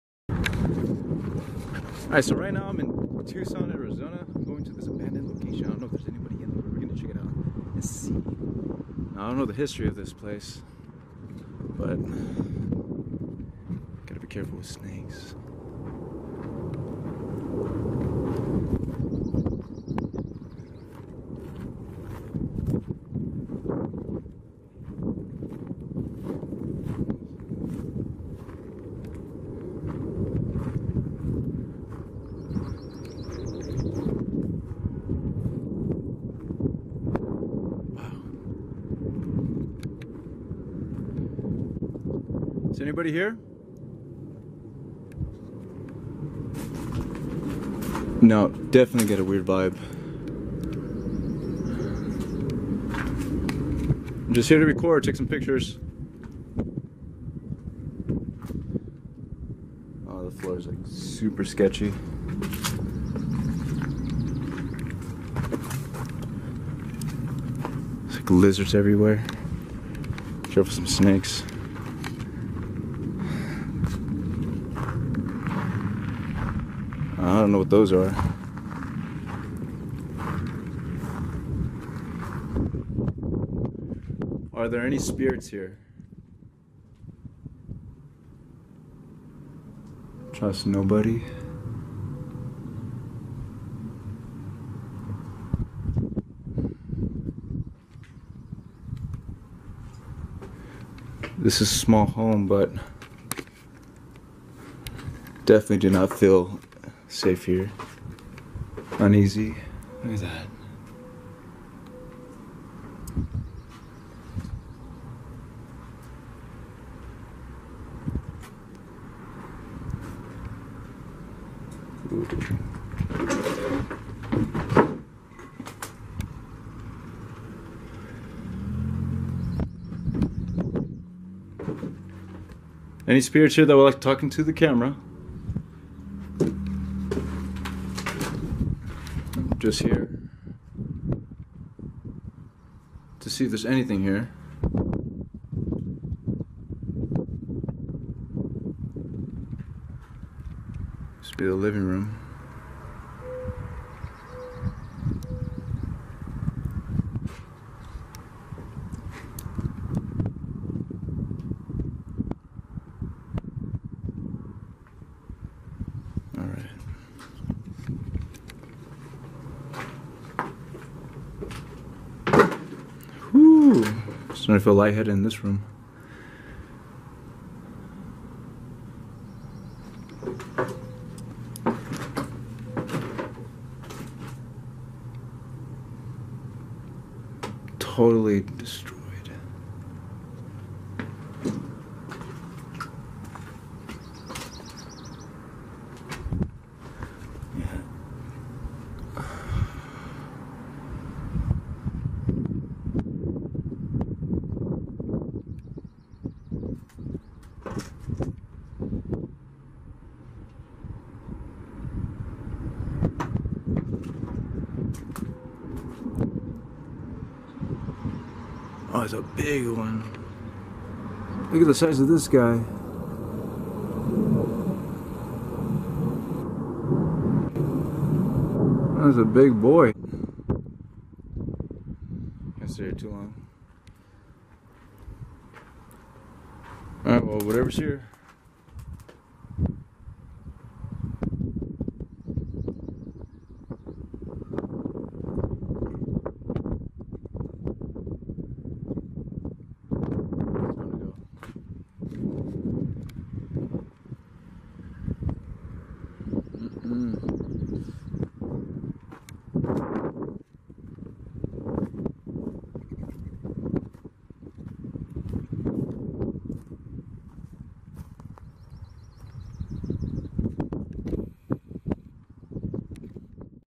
Broken down haunted shack!
While vacationing in Arizona, I couldn't resist exploring a suspicious-looking shack. Though the location may be modest, it certainly doesn't deter the presence of the paranormal from making itself known.

Thank you all for stopping by and checking out our video's! We appreciate you!

Carlos V.

The Calling
Spiritual Conflict